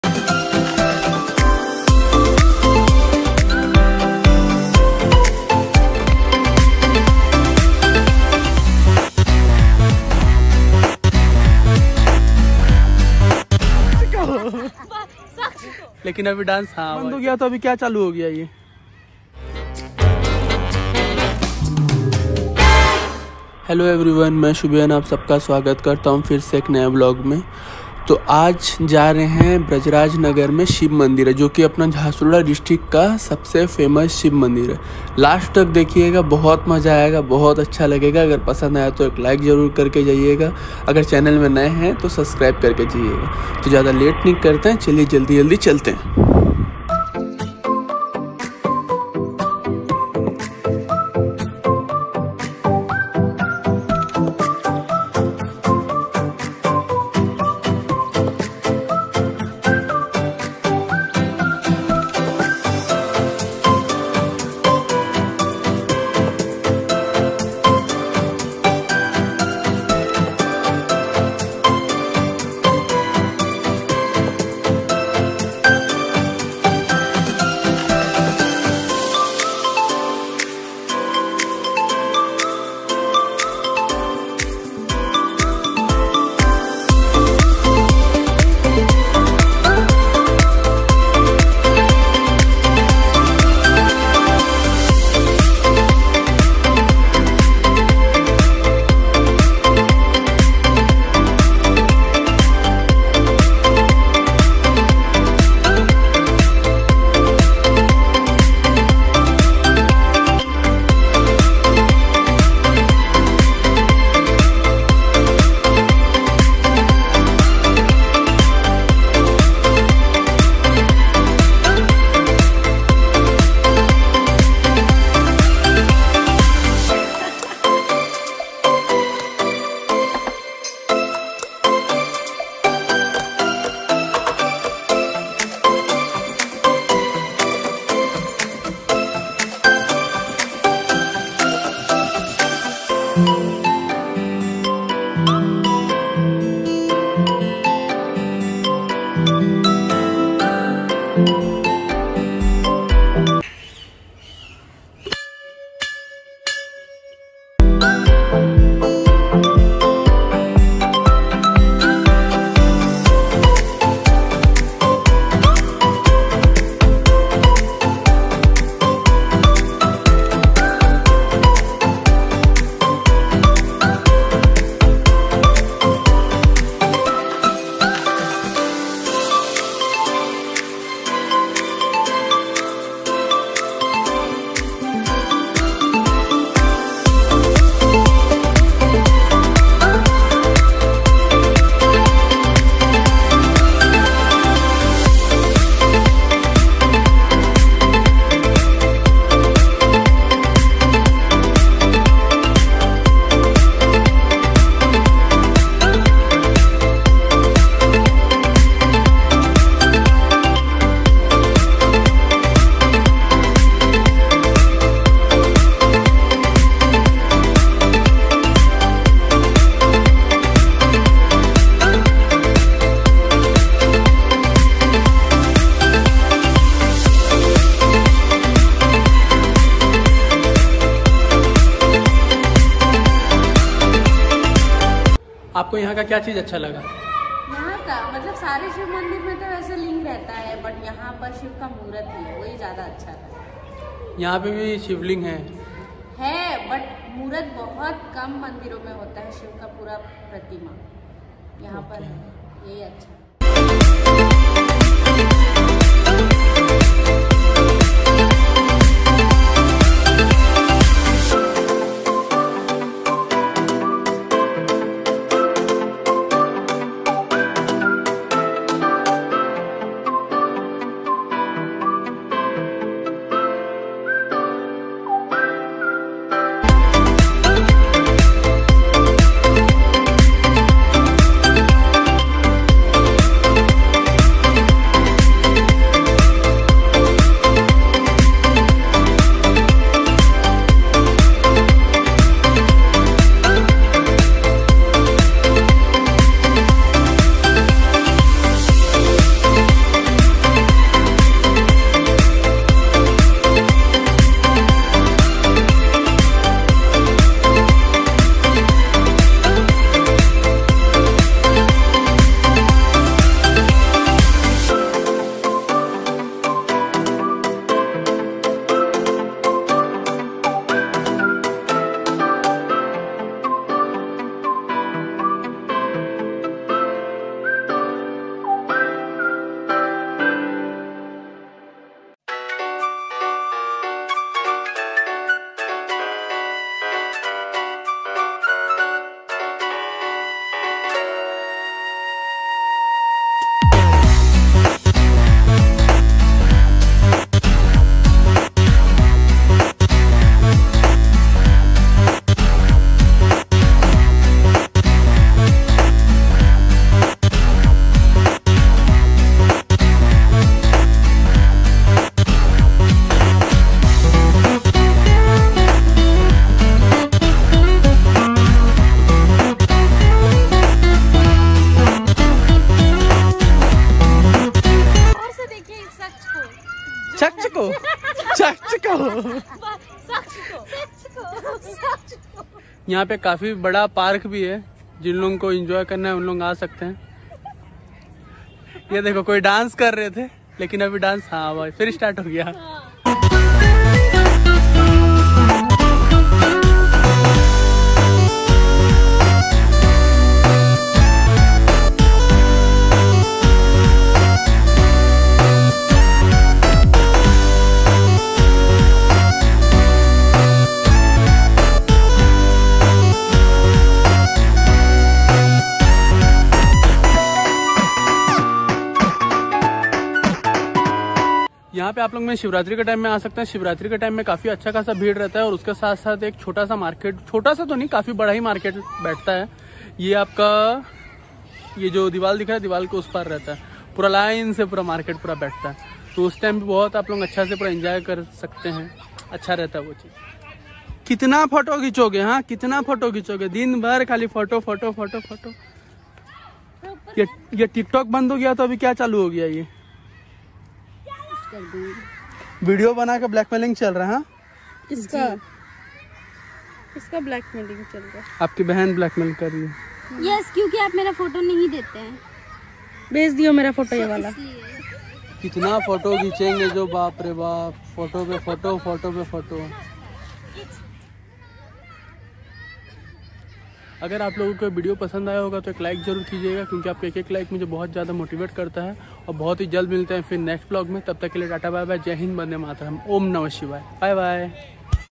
Shiv Mandir Brajrajnagar
This temple {Shiv Mandir} is famous in Jharsuguda District. Brajrajnagar is my home town till teenage. Welcome 🙏 all of you to my home town Brajrajnagar. Visit once this temple when u are searching for tourist place of jharsuguda district.

Social Media☺️